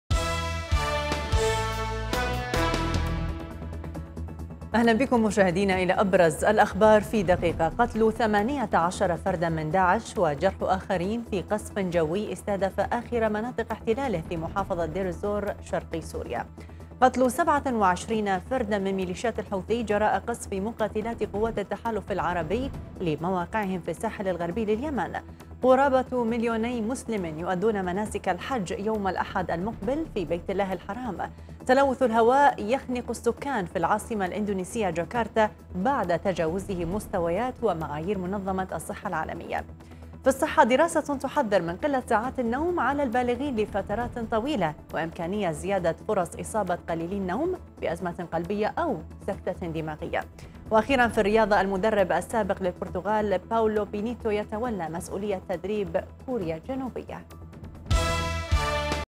دراسة تحذر من قلة ساعات النوم على البالغين لفترات طويلة - الأخبار بدقيقة
رمضان على تلفزيون الآن ٢٠١٩
شاهد آيضا:
قيادي بعثي يفضح تاريخ آل الأسد!؟

قتلُ 18 فردا من داعش وجرحُ اخرين في قصف جوي استهدف آخر مناطق احتلاله في محافظة دير الزور شرقي سوريا ...

قتل  27 فردا من ميليشيات الحوثي ، جراءَ قصفِ مقاتلاتِ قوات التحالف العربي لمواقعهم في الساحل الغربي لليمن...

قرابة ُ مليوني مسلم يؤدون مناسك الحج يوم الأحد المقبل في بيت الله الحرام  ...

تلوث الهواء يَخنق السكان في العاصمة الإندونيسية جاكرتا بعد تجازوه مستوياتِ ومعايير منظمة الصحة العالمية ...

في الصحة ،
دراسة  تحذر من قلة ساعات النوم على البالغين لفترات طويلة ، وامكانية زيادة فرص ِ اصابة قليلي النوم بأزمة قلبية أو سكتة دماغية  ..

واخيرا في الرياضة

المدرب السابق للبرتغال باولو بينتو يتولى مسؤولية تدريب كوريا الجنوبية

برقية_أخبار_الآن تقديم: روناك عادل